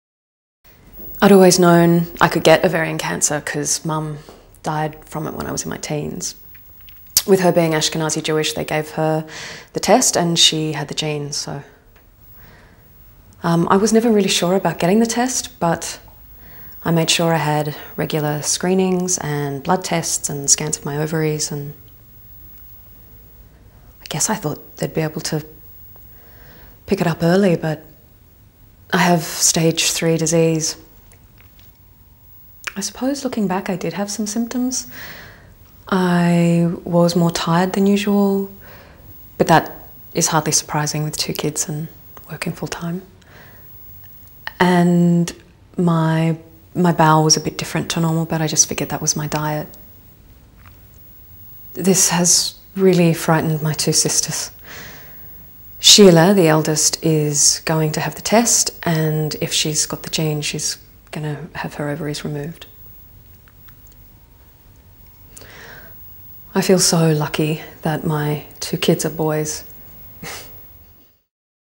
Jane's story 1
Jane is a 36-year-old female who was recently diagnosed with ovarian cancer.